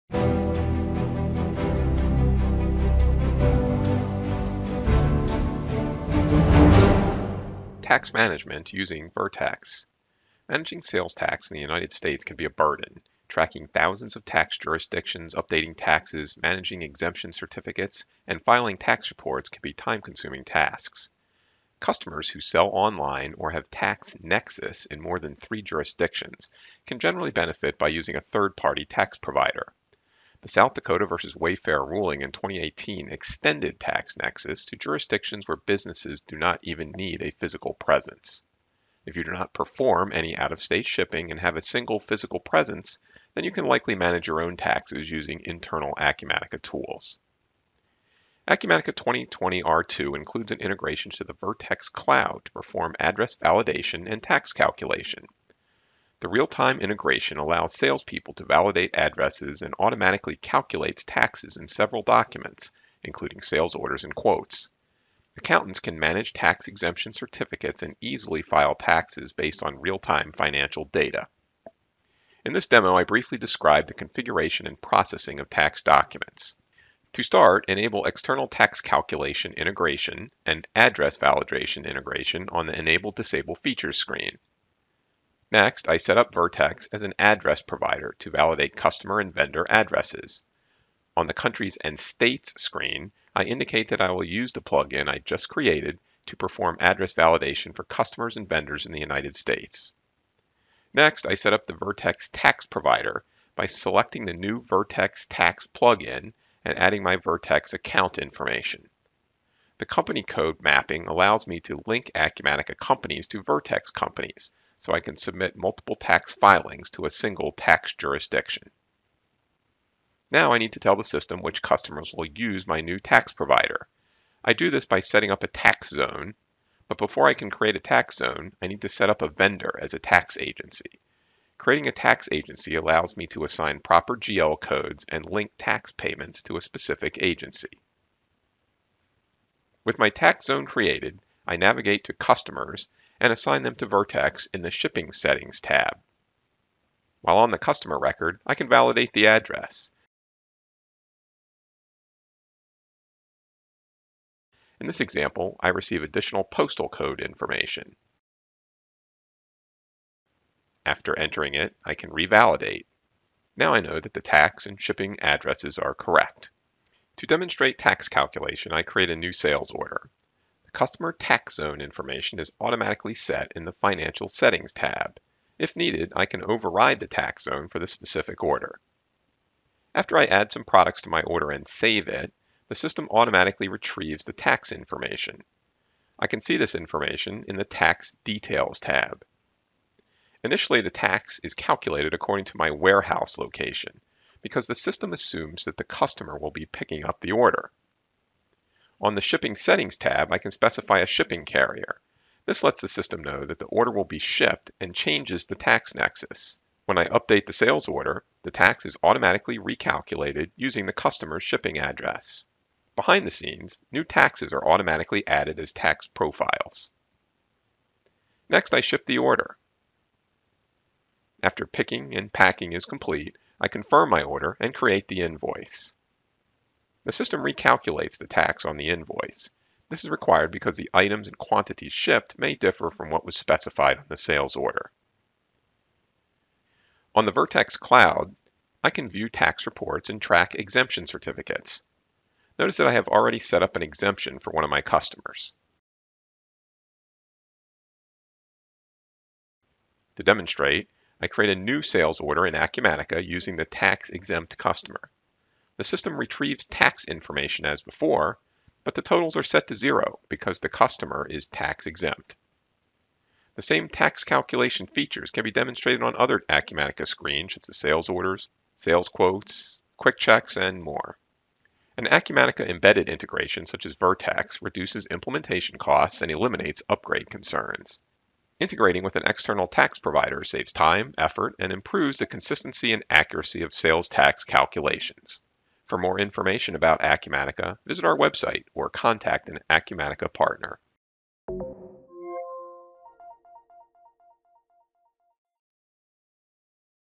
Acumatica Tax Management using the Vertex Tax Cloud
Learn how to manage sales taxes and validate addresses in Acumatica using an external tax provider such as Vertex.
Acumatica 2020 R2 demo showing Vertex setup plus examples of integrated address validation, tax computation, and tax exempt customer management.